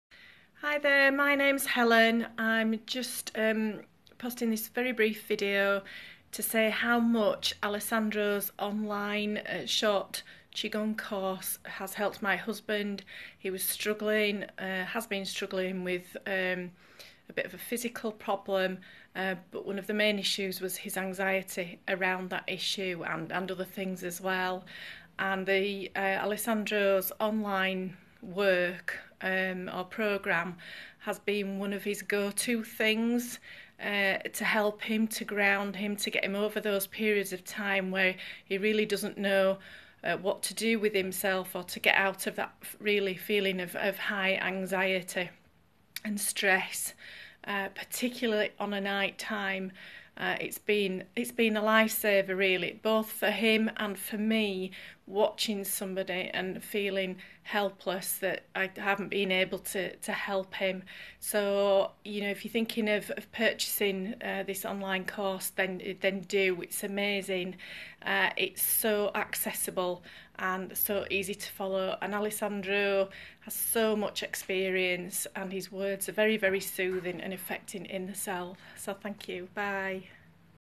Qigong testimonial for online courses, easy to follow in the comfort of your own home/office.
Online courses are available to follow at your own pace.
here is the link for a series of 6 videos focussed on self healing . can also arrange live (1 to 1) sessions or group work in English, Italian, French or German.